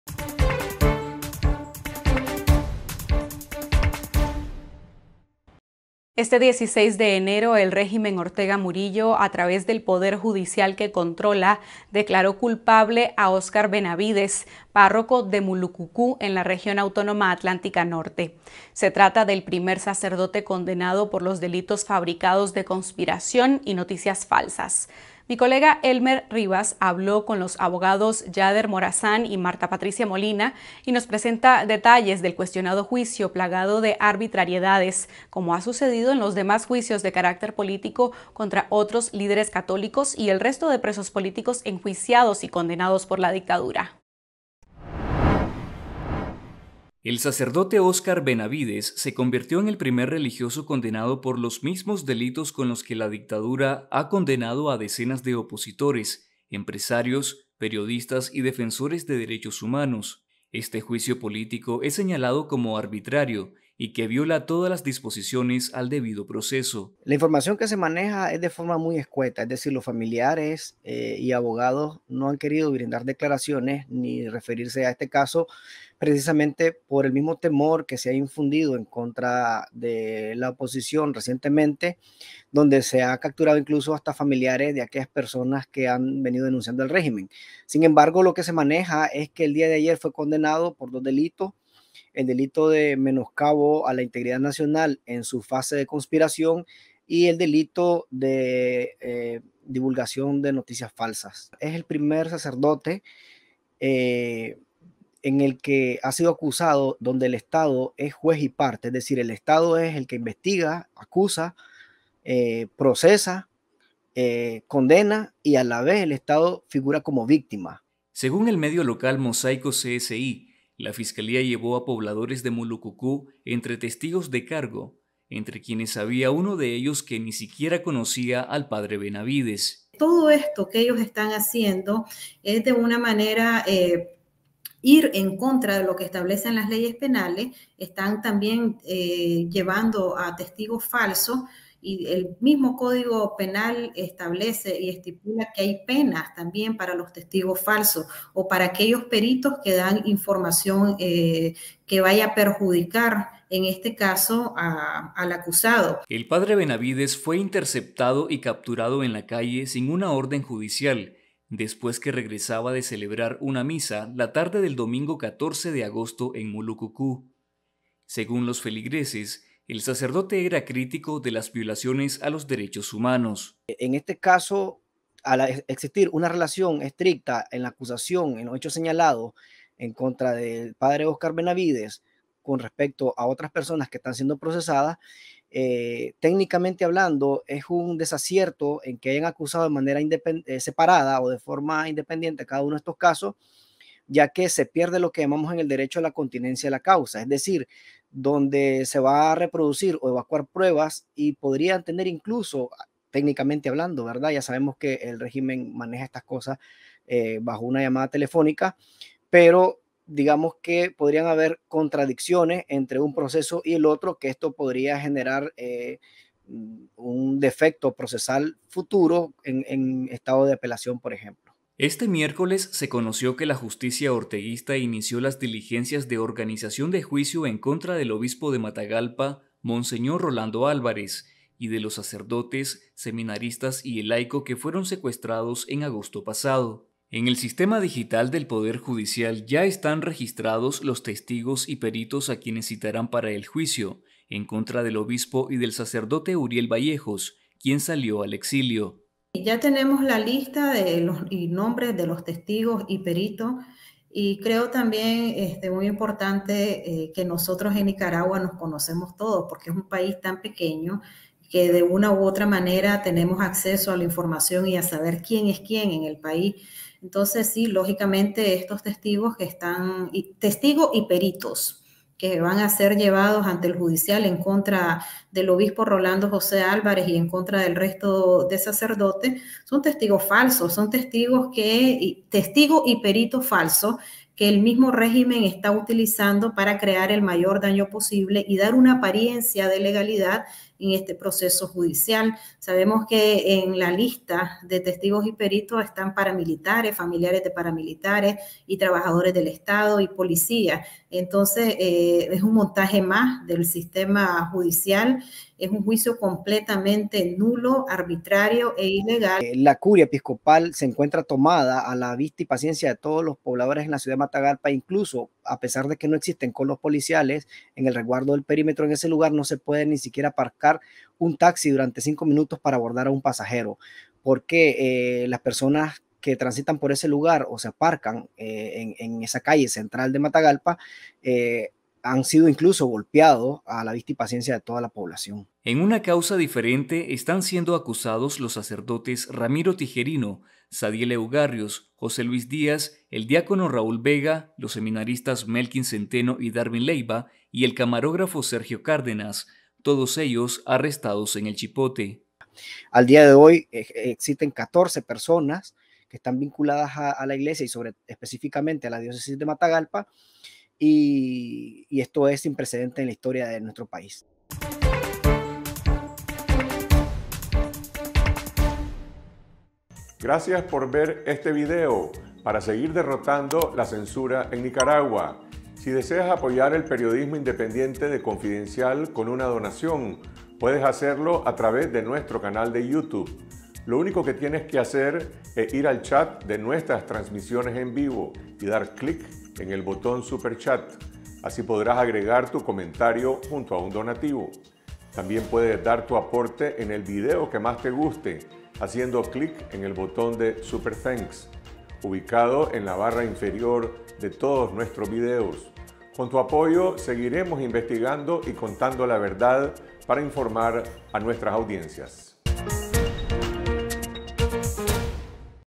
Oscar Benavides: el primer sacerdote condenado por "conspiración"
El exfuncionario judicial, Yader Morazán, y la defensora de derechos humanos, Martha Patricia Obando, analizan las violaciones al debido proceso y las repercusiones de la condena del sacerdote Oscar Benavides, el primer religioso en recibir una pena carcelaria por los supuestos delitos de "conspiración para cometer menoscabo a la integridad nacional y divulgación de noticias falsas".
Benavides, párroco de Mulukukú, en el Caribe Norte nicaragüense, ha sido crítico de los atropellos del régimen de Daniel Ortega y Rosario Murillo, lo que le habría valido la persecución de la dictadura, misma que viven otros sacerdotes, obispos y colaboradores de la iglesia católica de Nicaragua.